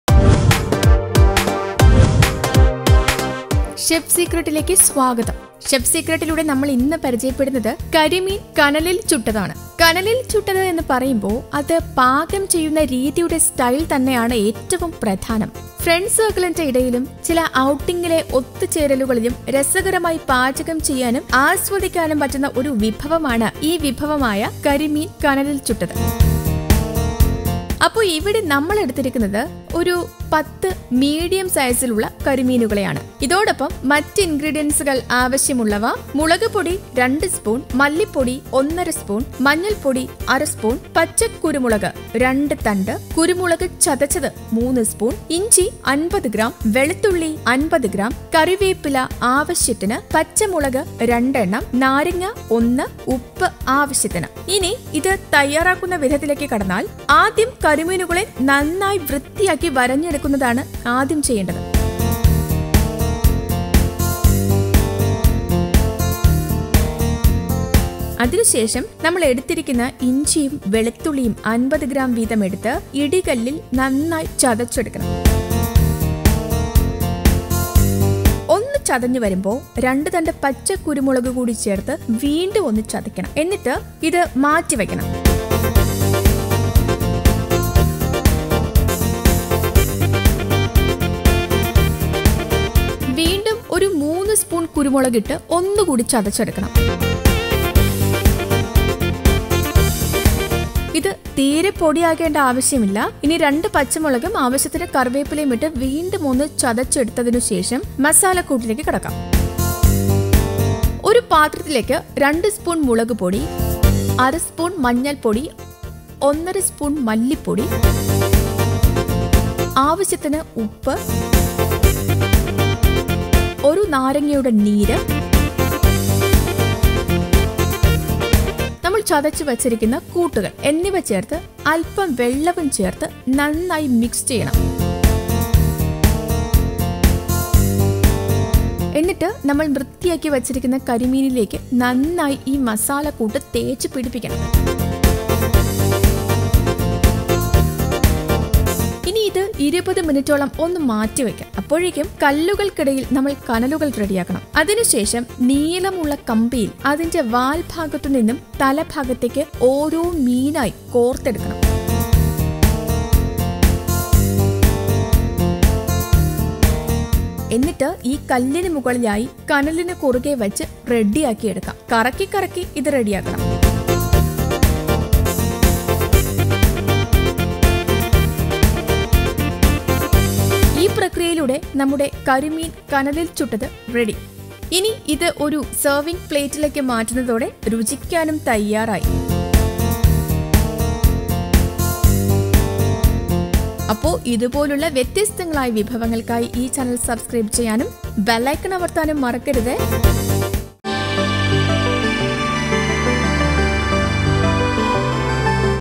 കരിമീന്‍ കനലില്‍ ചുട്ടത് | Tasty Grilled Fish | Chef's Secret
മുളക്‌പൊടി - 2സ്പൂണ്‍
മല്ലിപ്പൊടി- ഒന്നര സ്പൂണ്‍
മഞ്ഞള്‍പ്പൊടി- അര സ്പൂണ്‍
പച്ച കുരുമുളക് - 2 തണ്ട്
കുരുമുളക് ചതച്ചത് - 3 സ്പൂണ്‍
ഇഞ്ചി - 50 ഗ്രാം
വെളുത്തുള്ളി - 50 ഗ്രാം
 കറിവേപ്പില - ആവശ്യത്തിന്
പച്ചമുളക് - 2 എണ്ണം
 നാരങ്ങ - 1
ഉപ്പ് - ആവശ്യത്തിന്